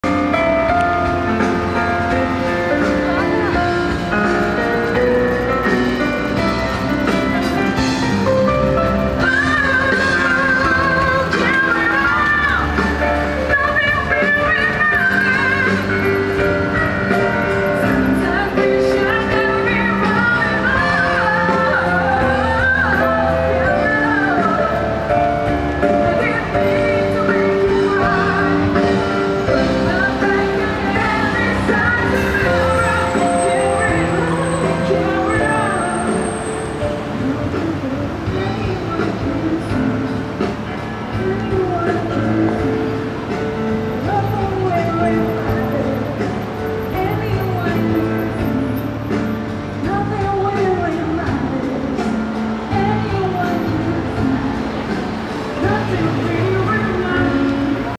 Fukuoka - Jazz Festival
Un peu du "Jazz Spirit" soufflant sur Fukuoka lors de mon passage dans la ville...